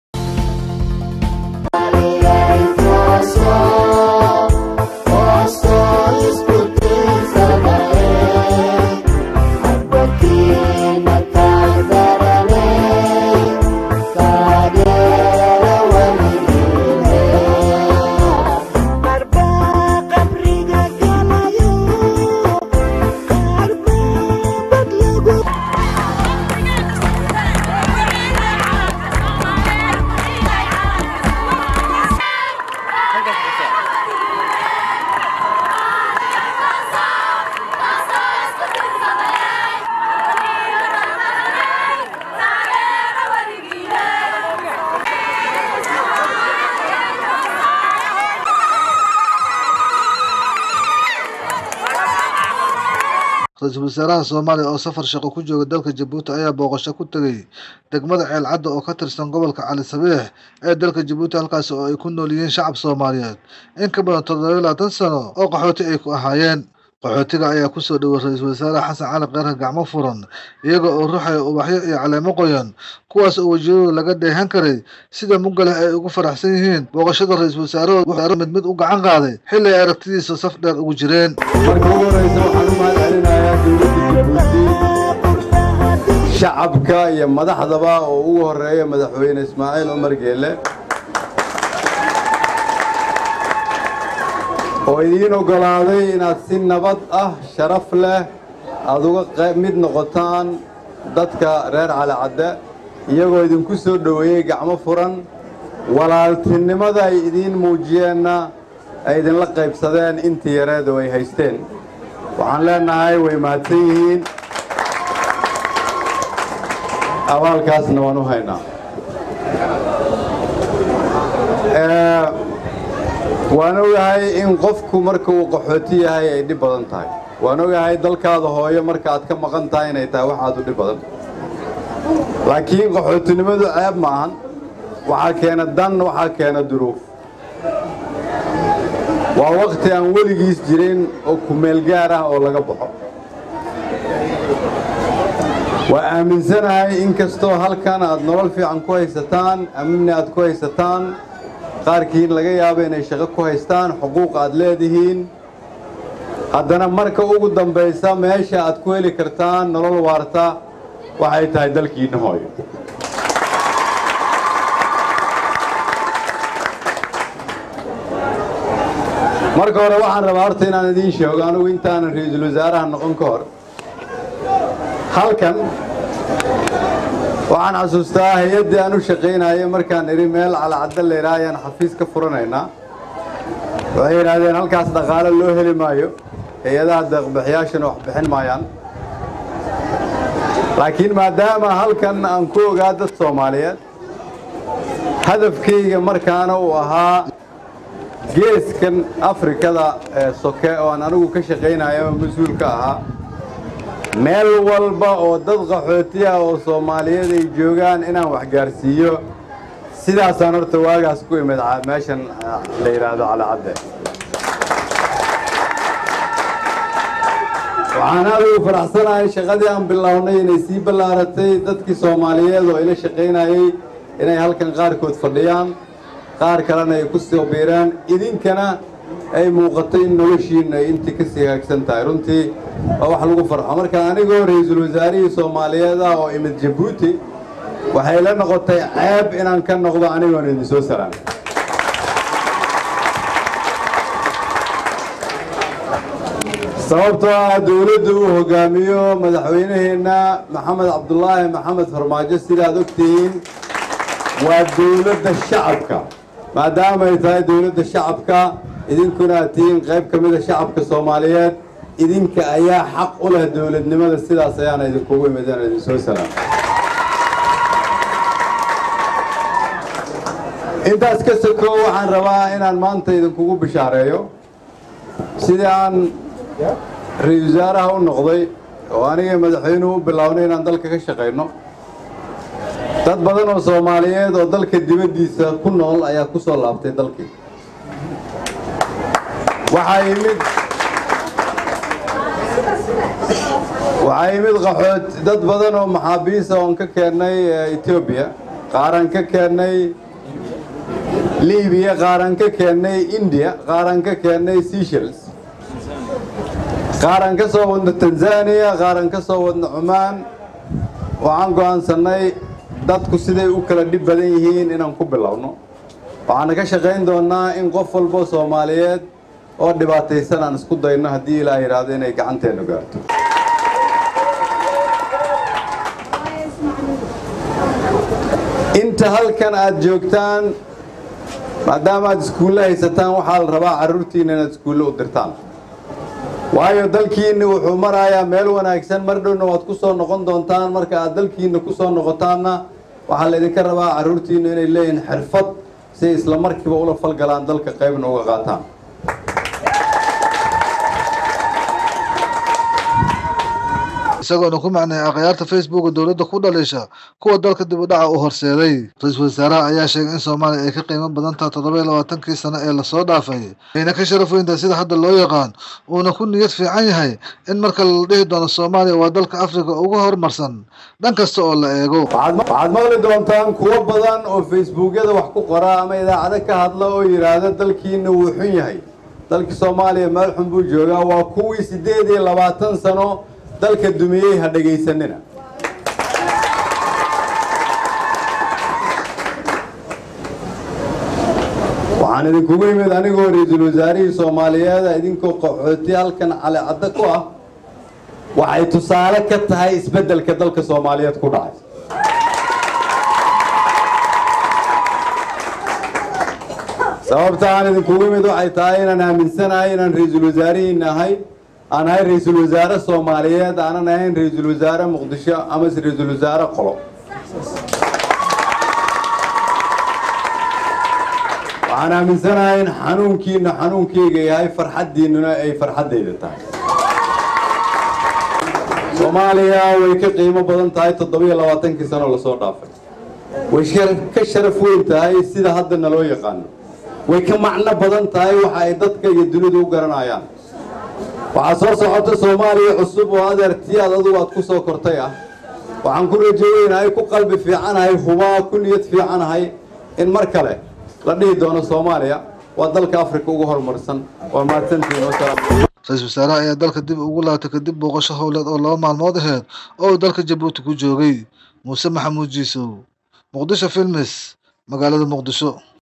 Cali cadde gobolka Cali Sabiix ee dalka Jabuuti
Qaxootiga ayaa ku soo dhaweeyey RW  Kheyre Gacmo furan iyada oo luxayey Ubax iyo Caleemo Qoayan kuwaas oo wajiyadooda laga dheehan karo sida muga leh ee ay ugu faraxsan yihiin booqashada WRahooda wuxuuna mid mid u gacan qaaday xili ay aragtiisa safn dheer ugu jireen